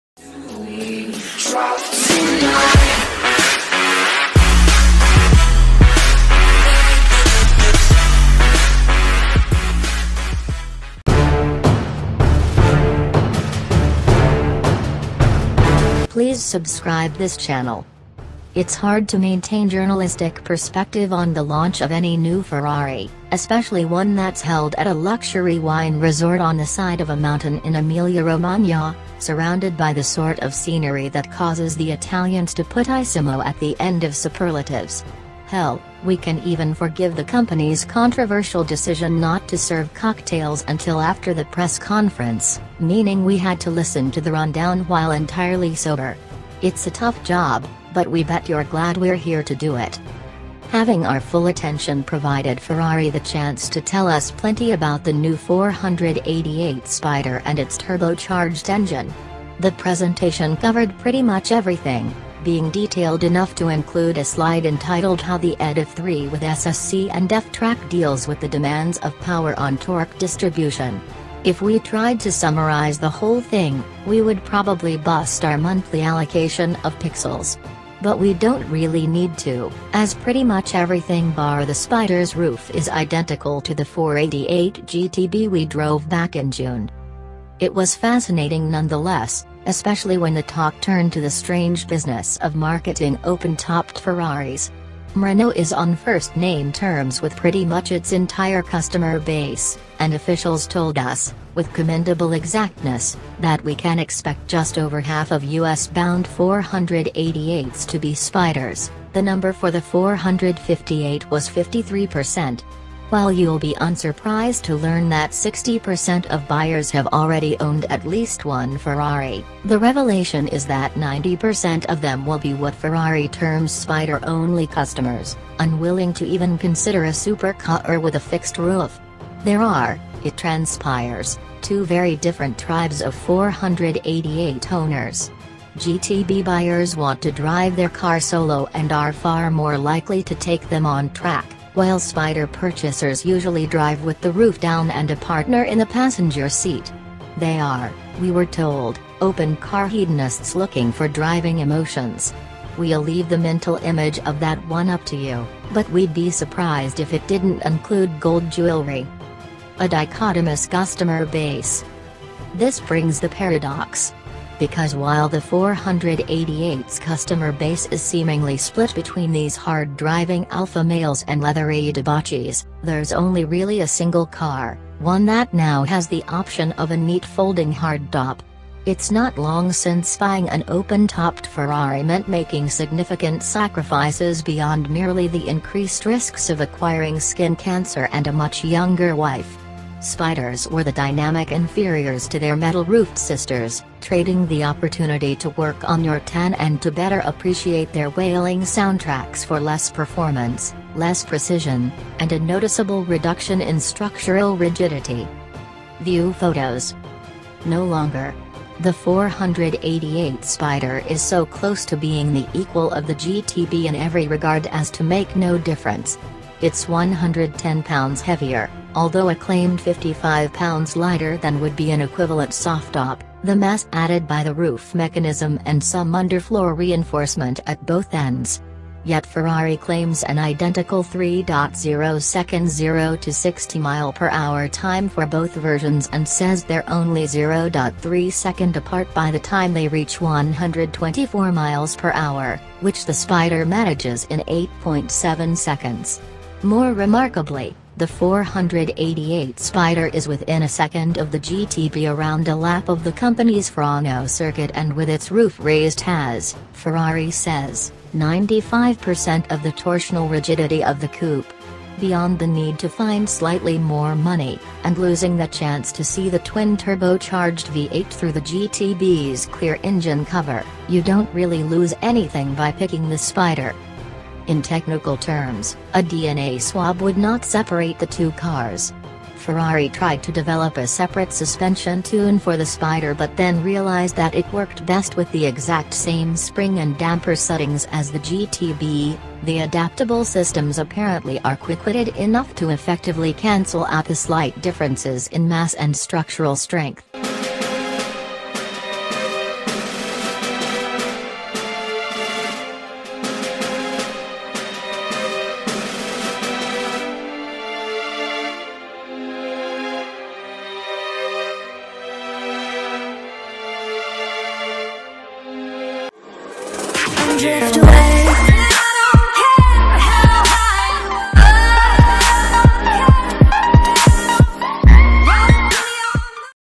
[ WATCH ] 2016 Ferrari 488 Spider Review
2016 Ferrari 488 Spider Review. It’s hard to maintain journalistic perspective on the launch of any new Ferrari, especially one that’s held at a luxury wine resort on the side of a mountain in Emilia Romagna, surrounded by the sort of scenery that causes the Italians to put “issimo” at the end of superlatives. Hell, we can even forgive the company’s controversial decision not to serve cocktails until after the press conference, meaning we had to listen to the rundown while entirely sober. It’s a tough job, but we bet you’re glad we’re here to do it.

Having our full attention provided Ferrari the chance to tell us plenty about the new 488 Spider and its turbocharged engine. The presentation covered pretty much everything, being detailed enough to include a slide entitled “How the Ediff3 with SSC and F-trac deals with the demands of power-on torque distribution.” If we tried to summarize the whole thing, we would probably bust our monthly allocation of pixels. But we don’t really need to, as pretty much everything bar the Spider’s roof is identical to the 488GTB we drove back in June.

It was fascinating nonetheless, especially when the talk turned to the strange business of marketing open-topped Ferraris. Maranello is on first-name terms with pretty much its entire customer base, and officials told us, with commendable exactness, that we can expect just over half of U.S.-bound 488s to be Spiders (the number for the 458 was 53 percent). While you’ll be unsurprised to learn that 60 percent of buyers have already owned at least one Ferrari, the revelation is that 90 percent of them will be what Ferrari terms “Spider-only” customers, unwilling to even consider a supercar with a fixed roof. There are, it transpires, two very different tribes of 488 owners. GTB buyers want to drive their car solo and are far more likely to take them on track, while Spider purchasers usually drive with the roof down and a partner in the passenger seat. They are, we were told, “open-car hedonists looking for driving emotions.” We’ll leave the mental image of that one up to you, but we’d be surprised if it didn’t include gold jewelry.

A Dichotomous Customer Base

This brings the paradox. Because while the 488’s customer base is seemingly split between these hard-driving Alpha males and leathery debauchees, there’s only really a single car, one that now has the option of a neat folding hardtop. It’s not long since buying an open-topped Ferrari meant making significant sacrifices beyond merely the increased risks of acquiring skin cancer and a much younger wife. Spiders were the dynamic inferiors to their metal-roofed sisters, trading the opportunity to work on your tan and to better appreciate their wailing soundtracks for less performance, less precision, and a noticeable reduction in structural rigidity.